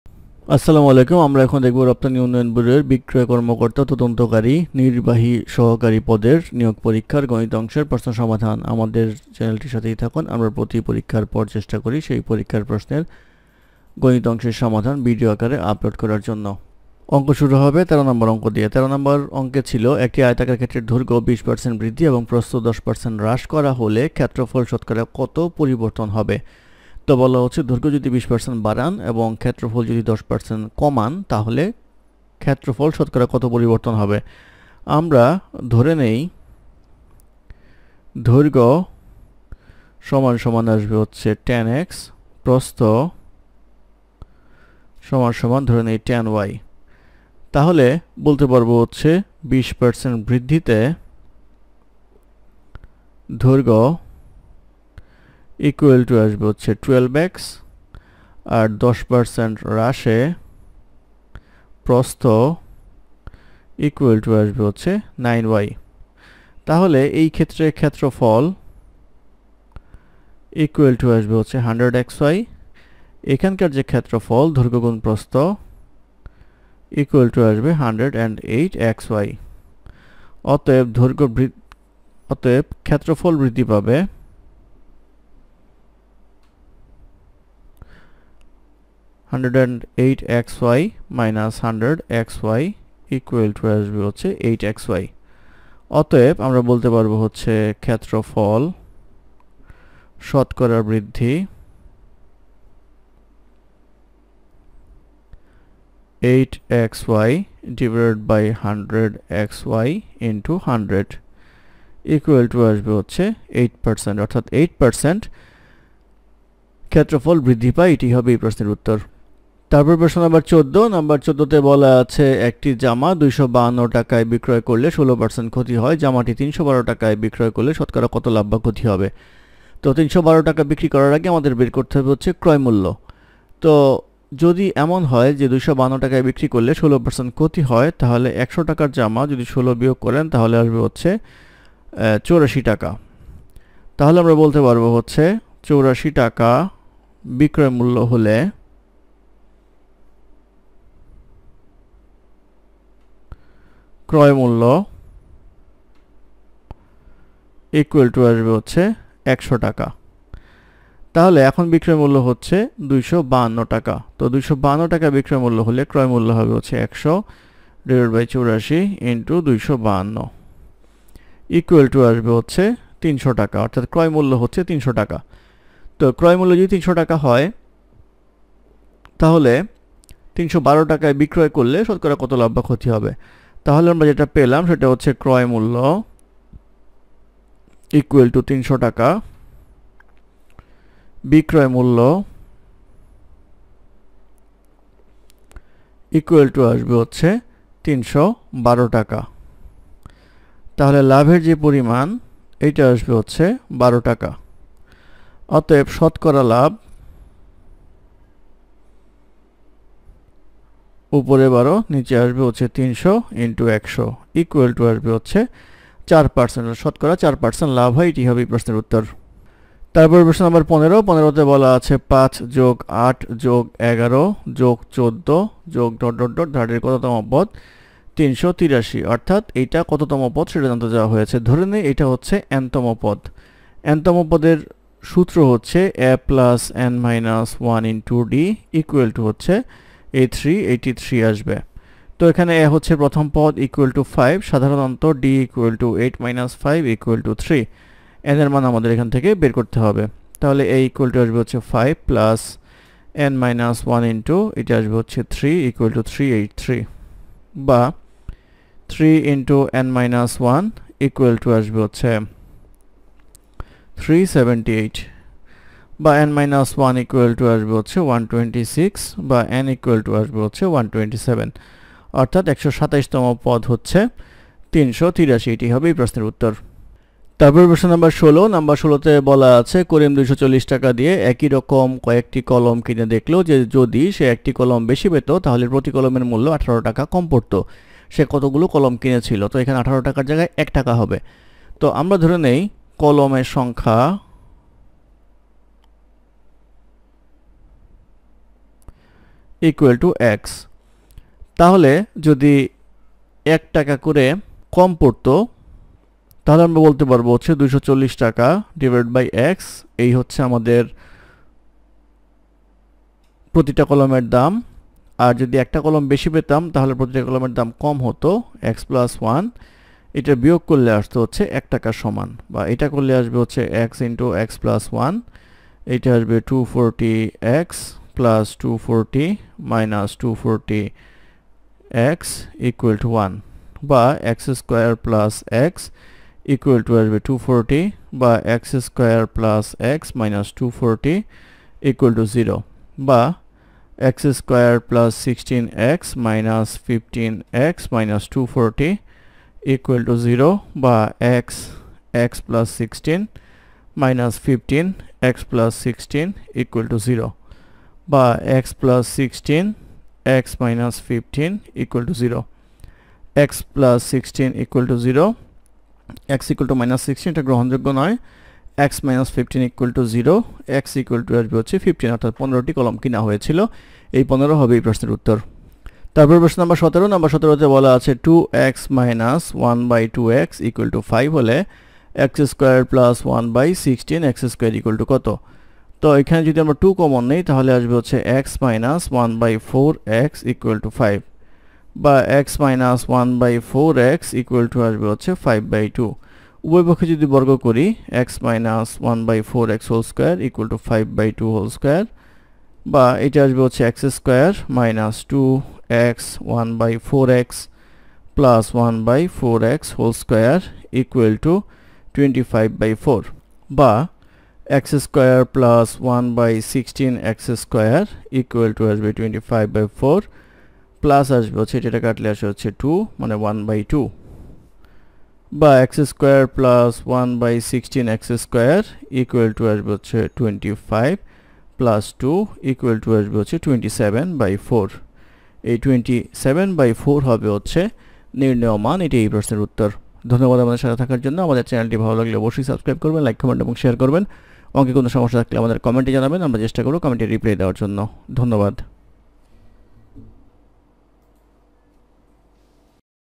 রপ্তানি উন্নয়ন ব্যুরো পদঃ বিক্রয় কর্মকর্তা/ তদন্তকারী/ নির্বাহী Written Math Exam Date: 01-07-2022
রপ্তানি উন্নয়ন ব্যুরো
পদঃ বিক্রয় কর্মকর্তা/ তদন্তকারী/ নির্বাহী সহকারী

১৩. একটি আয়তাকার ক্ষেত্রের দৈর্ঘ্য ২০% বৃদ্ধি এবং প্রস্থ ১০% হ্রাস করা হলে, ক্ষেত্রফল শতকরা কত পরিবর্তন হবে?
১৪. একটি জামা ২৫২ টাকায় বিক্রয় করলে ১৬% ক্ষতি হয়। জামাটি ৩১২ টাকায় বিক্রয় করলে শতকরা কত লাভ বা ক্ষতি হবে?
১৫. ৫+ ৮+ ১১+ ১৪ + ধরাটির কততম পথ ৩৮৩?
১৬. করিম ২৪০ টাকা দিয়ে একই রকম কয়েকটি কলম কিনে দেখল যে, যদি সে একটি কলম বেশি পেত তাহলে প্রতিটি কলমের মূল্য ১ টাকা কম পড়তো। সে কতগুলো কলম কিনেছিল?
১৭. 2x – 1/2x = 5 হলে, x2 + 1/ 16x2  এর মান নির্ণয় করুন।